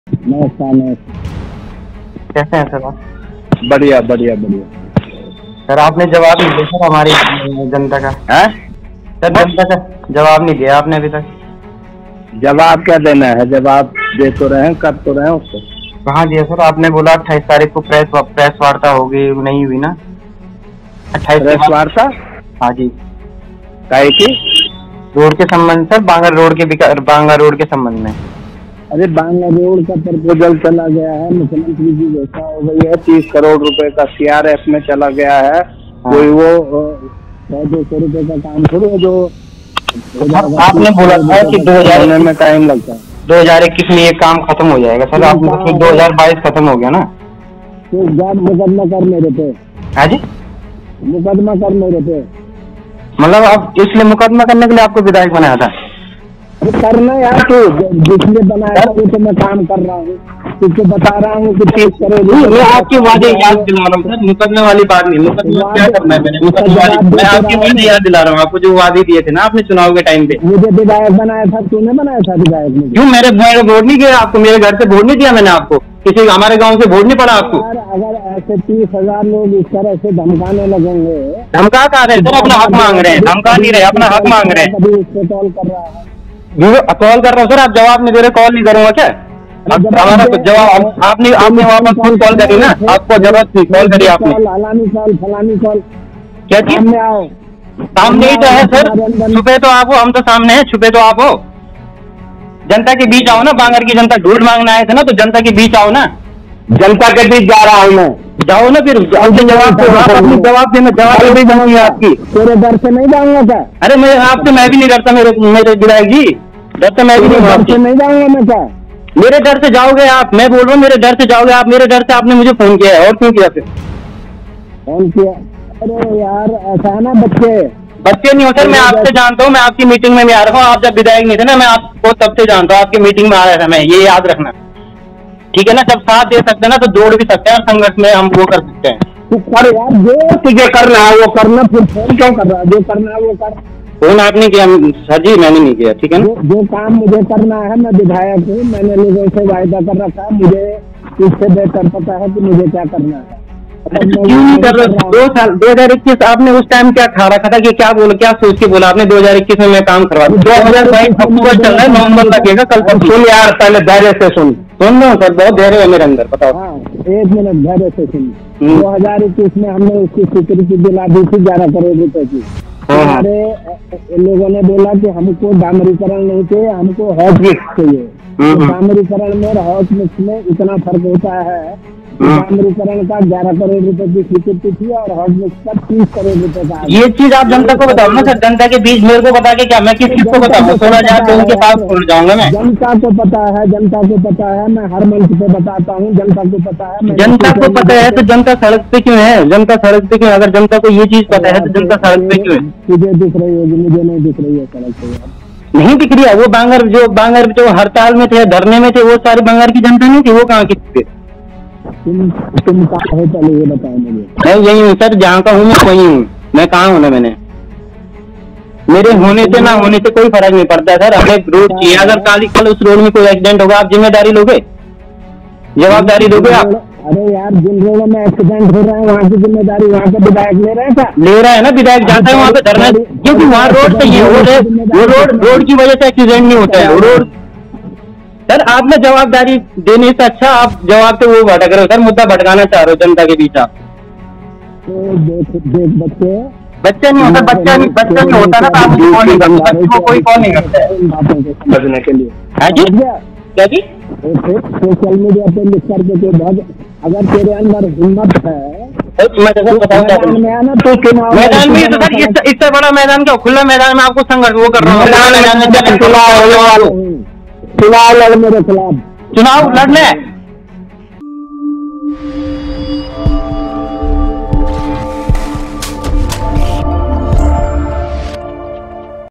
विधायक का ऑडियो वायरल : बोले -तूने बनाया था मुझे विधायक? जा मेरे पे मुकदमा क़र.मेरे खिलाफ चुनाव लड़.
खबर सार : मामला रुद्रप्रयाग विधानसभा का बताया जा रहा है जहाँ जखोली ब्लॉक के बांगर क्षेत्र में जनता सड्क के सुधारिकरण की लम्बे समय से माँग क़र रहीं है.आपको बता दें कि इस सड्क से प्रभावित 16 गाँवो के लोगो का कहना है कि चुनाव के टाइम पर विधायक भरत सिंह चौधरी द्वारा उनसे इस सड्क को हॉटमिक्स करवाने का वादा किया गया था जोकि अब तक पूरा नही हो पाया है. वहीं विधायक भरत सिंह चौधरी ऑडियो में कह भी रहे हैं कि सड्क के संबंध में प्रपोजल चला गया है. मुख्यमंत्री की घोषणा भी हो चुकी है, 30 करोड़ रूपये सीआरएफ में चला गया है कोई वो सौ दो सौ रूपये का काम थोड़े ही है होने में टाइम लगता है.

ऑडियो में स्थानीय युवा विधायक को उनके चुनावी वादे की याद दिला रहा है हालाँकि विधायक युवक को जानकारी भी दे रहे हैं कि सड्क के प्रपोजल की वास्तविक स्थिति क्या है?? लेकिन वहीं विधायक युवक की बातों से झुंझलाकर ये भी कहते हुए सुनाई दे रहे हैं कि जा मुकदमा क़र मेरे पे.
युवक कह रहा है कि क्या इसलिए हमने आपको विधायक बनाया था?

फिर युवक कहता है कि मै सिर्फ आपको आपके वादे याद दिला रहा हुँ. विधायक अत्यधिक झुंझुलाहट में कहते हैं कि --क्या तूने बनाया था मुझे विधायक??कुलमिलाकर विधायक की युवा से तीखी बहस शुरू हो जाती है.

कुलमिलाकर विधायक भरत सिंह चौधरी सड्क की वास्तविक स्थिति से युवा को वाकिफ तो जरूर करवा रहे हैं वहीं उनकी बातों में युवा के सवालों से झुंझलाहट भी दिख रही है जिस वजह से वो कुछ ऐसी बातें बोलते हुए नजर आ रहे हैं जिसकी वजह से ओड़ियो वायरल हो गया है और चर्चाओं का विषय बना हुआ है.

Produced By: Pahad tv
Edited By: swati negi